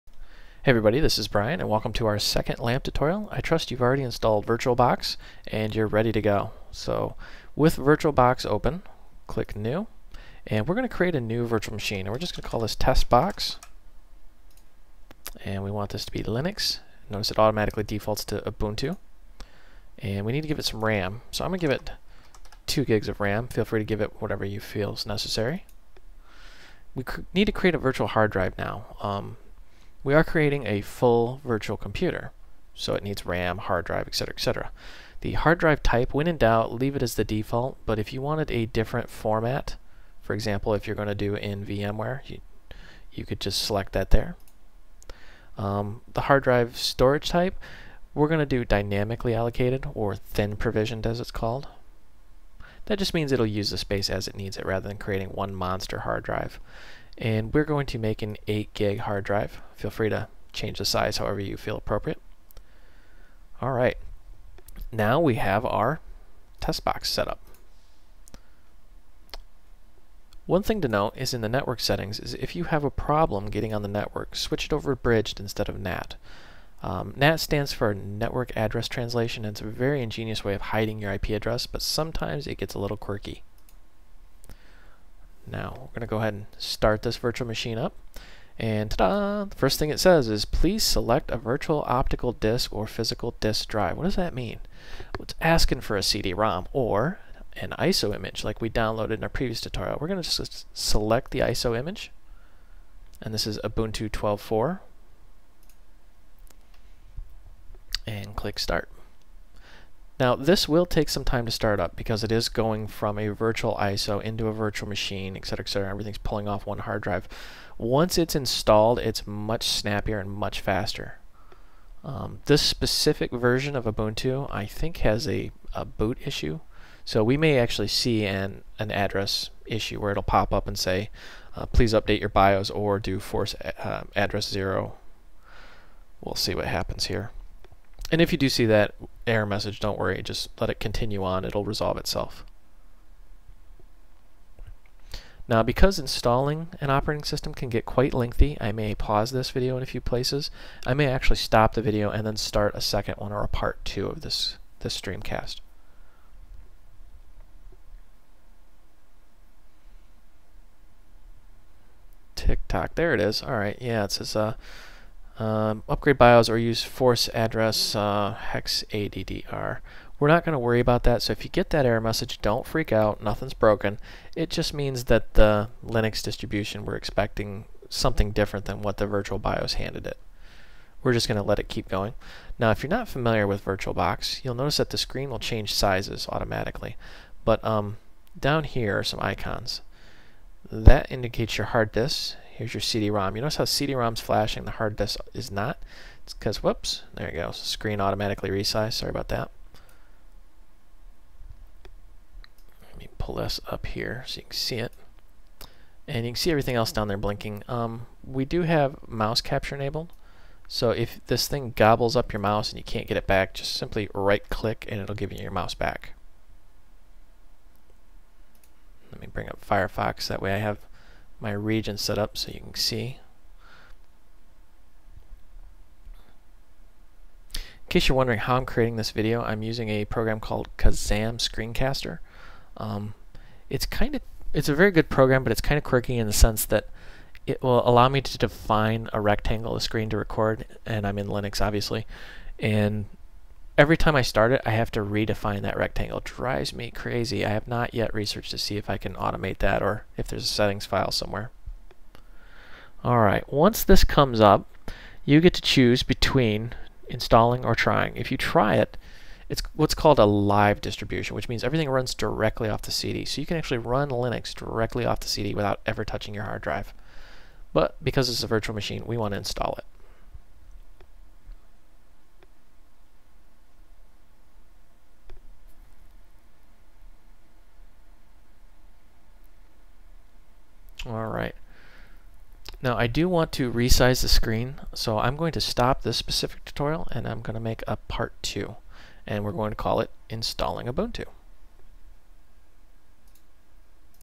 LAMP 2 - Setting up a virtual machine
Learn how to set up a virtual machine for these tutorials